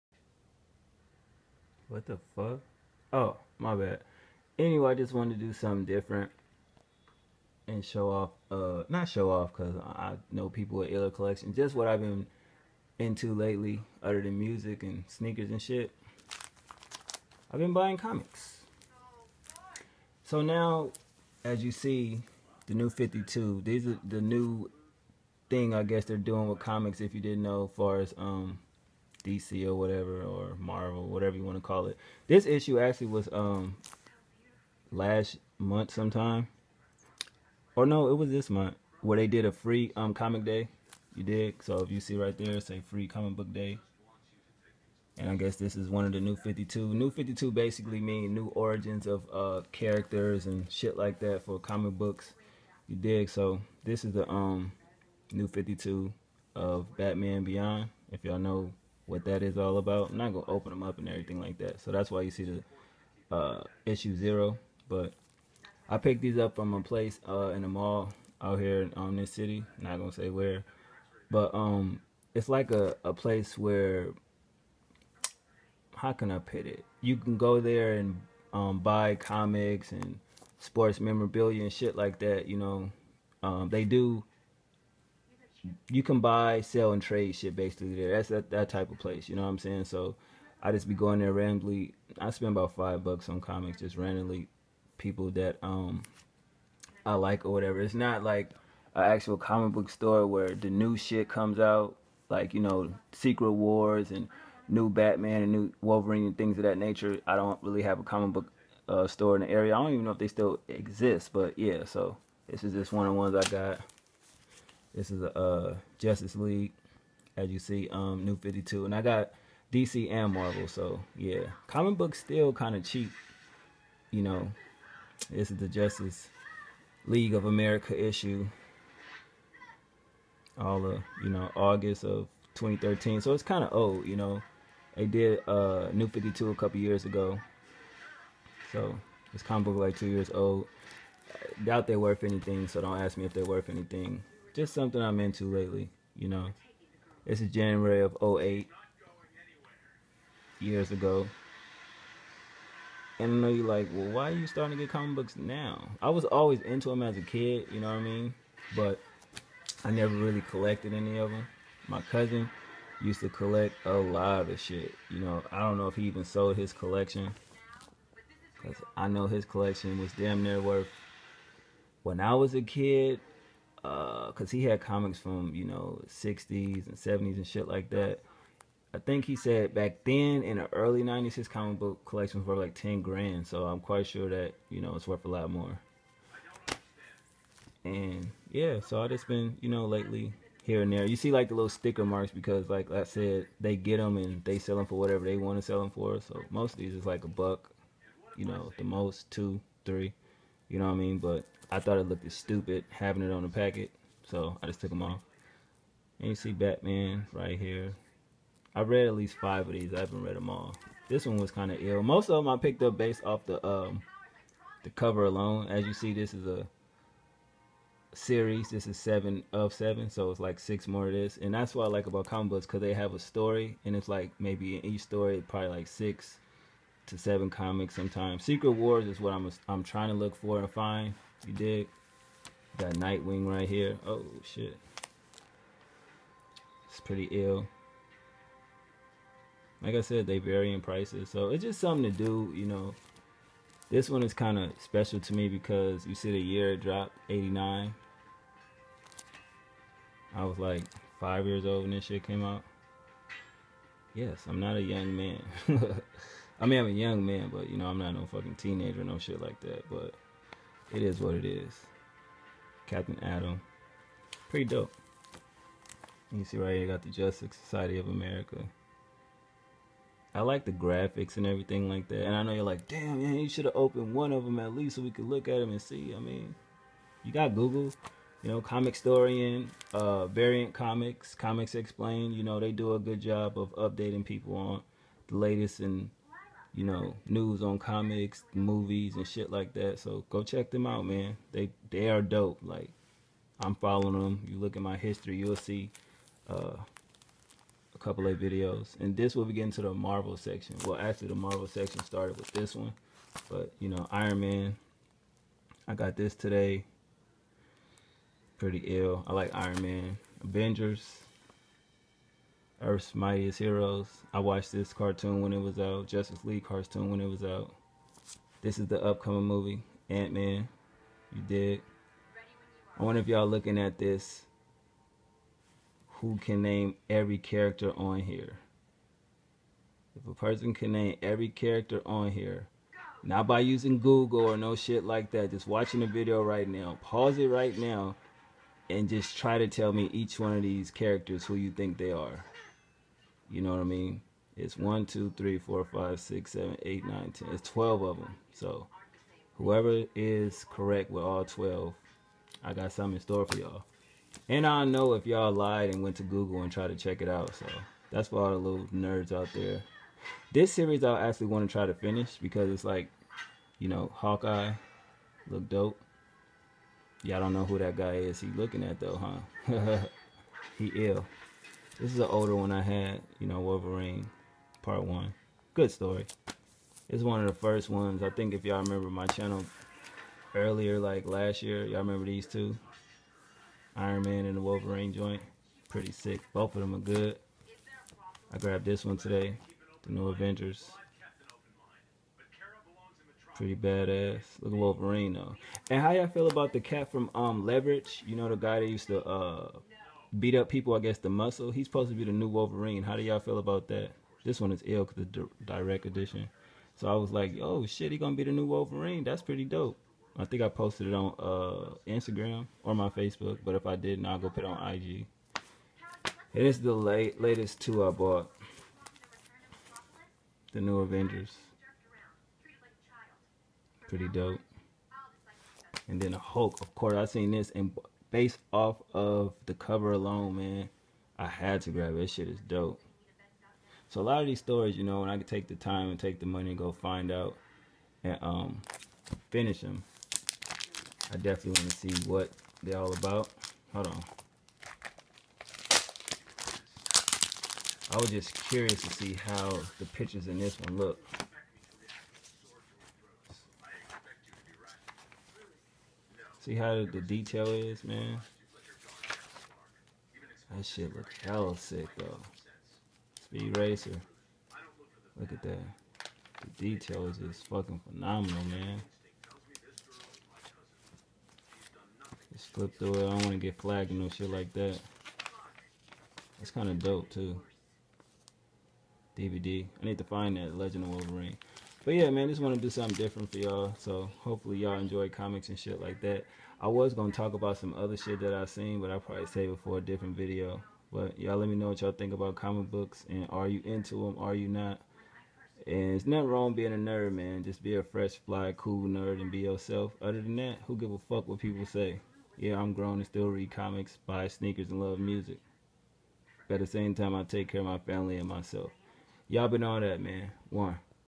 Random vlog: My Mini Comic Book Collection- Marvel & DC Comics...
Are any of you guys into comics? If so who do you like more 'Marvel or DC Comics'..?